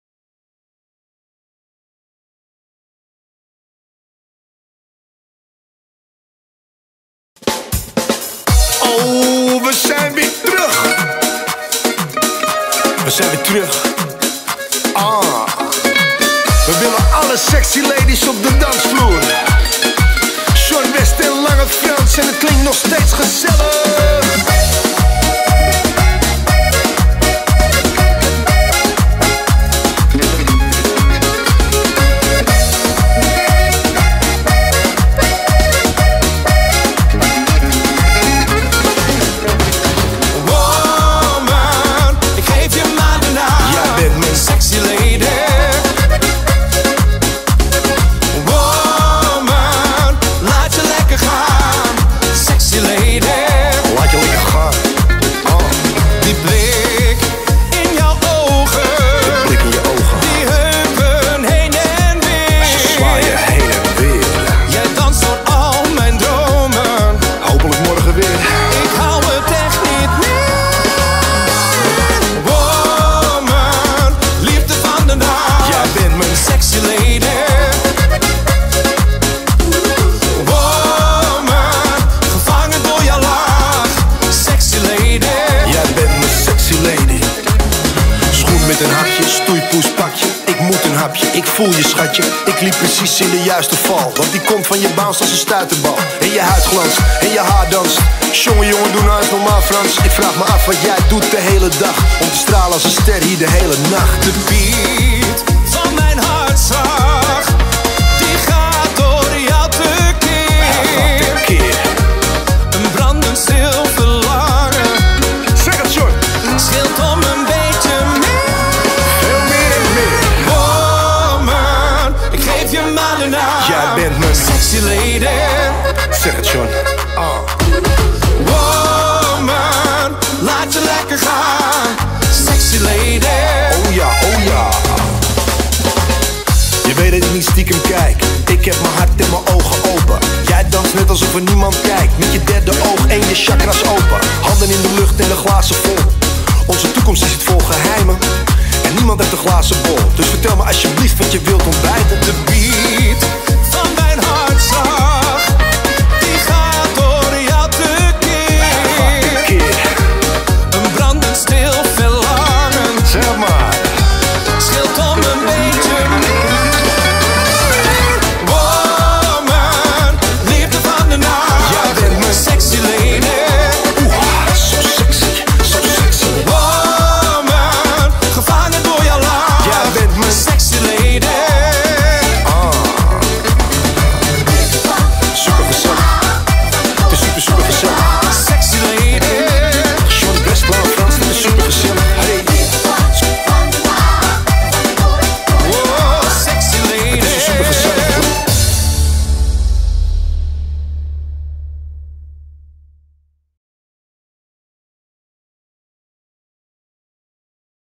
Sexy Lady /High Beginner - Line Dance
32c/4w  High Beginner
Choreo: Triple X, Raymond Sarlemijn (NL), Darren Bailey (UK) Roy Verdonk (NL) - January 2024